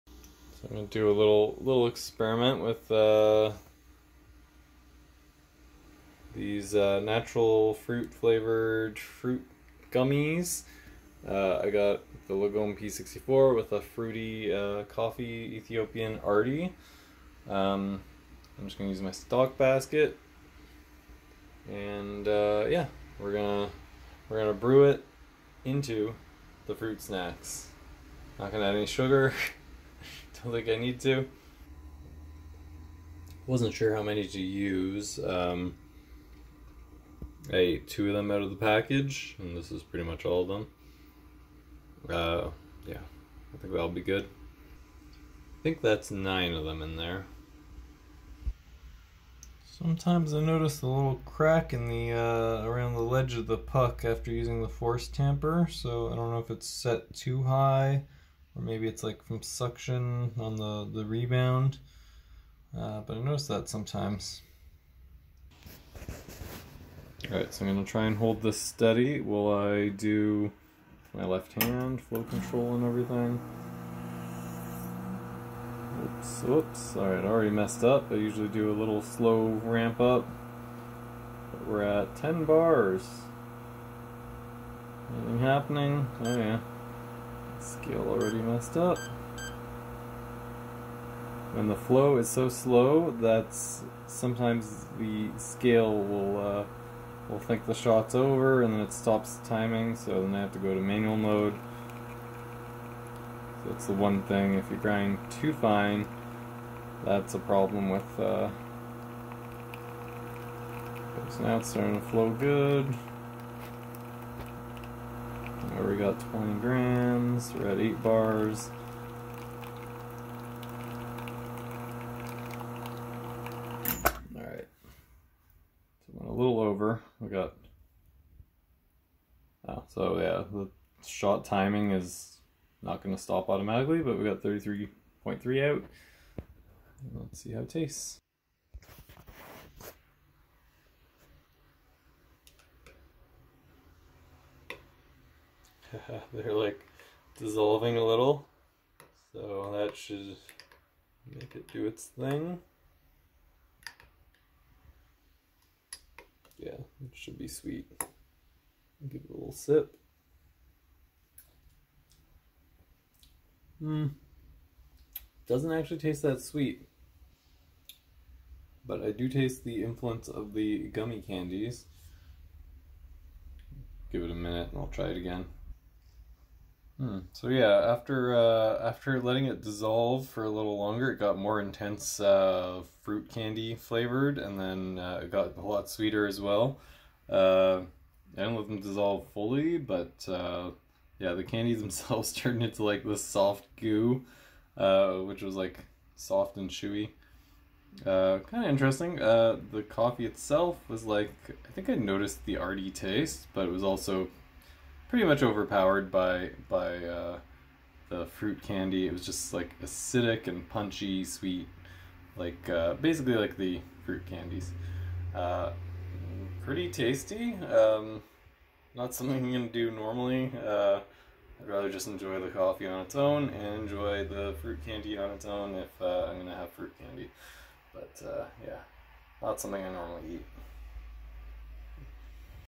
How to turn bad coffee into a nice fruity & sweet coffee?
I did actually start with good coffee in this case just ground too fine so not a perfect shot but still turned out good!